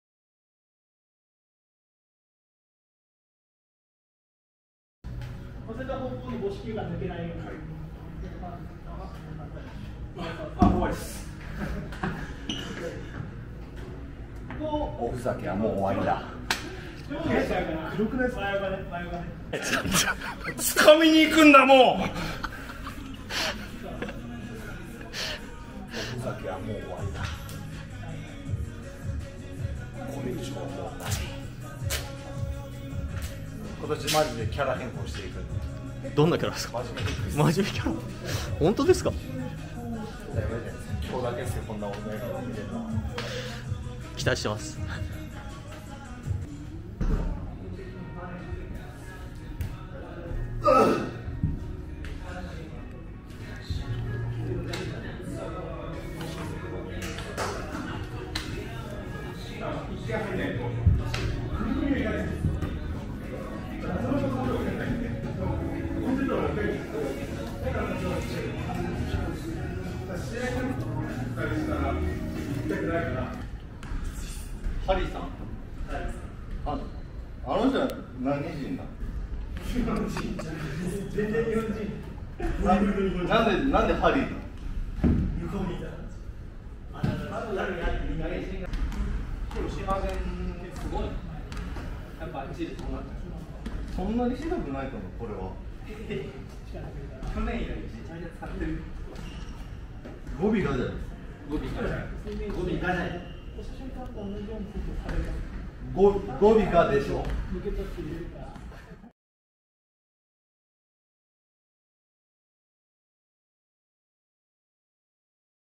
【今季もしっかり】7/11・12 小ネタまとめ（ショートバージョン）【盛りだくさん】
フルバージョンは「クラブハピネッツ」で公開中！

今季もあるのかい、ないのかい、どっちなんだい！あーる！パワー！チーム練習を開始した7月11日・12日の練習からピックアップした小ネタをお届け！

入会受付中！「クラブハピネッツ」とは？

 B.LEAGUE（B1東地区）
秋田ノーザンハピネッツ